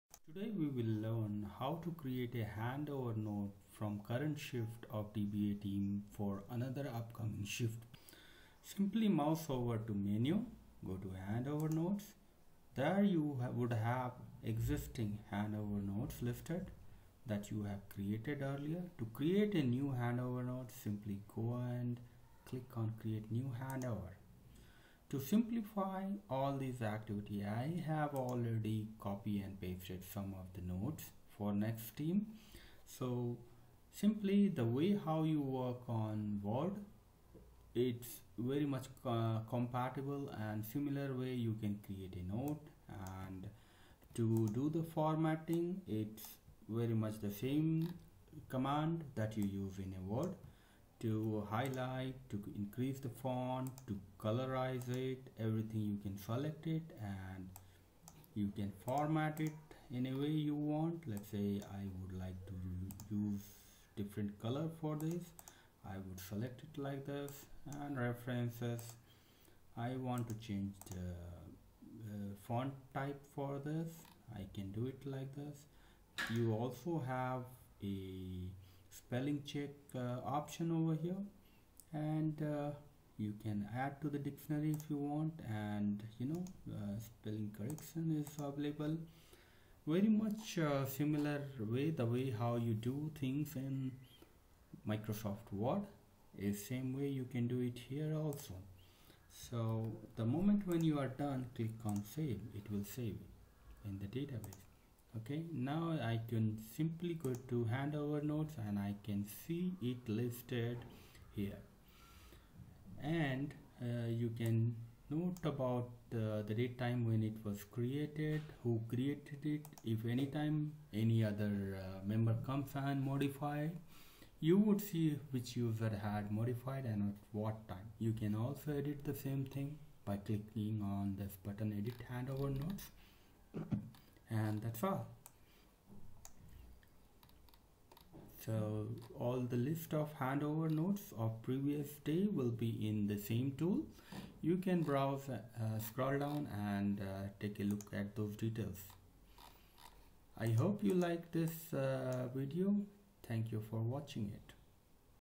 DBA Handover Notes Management
DBA Shift Handover notes management in SQL Planner
Are you (DBA) sharing handover notes through email and its is lost when it gets older , do you want to track it in better way and track all changes. SQL Planner has built in feature of this , watch demo here.

to know more about features or download visit to official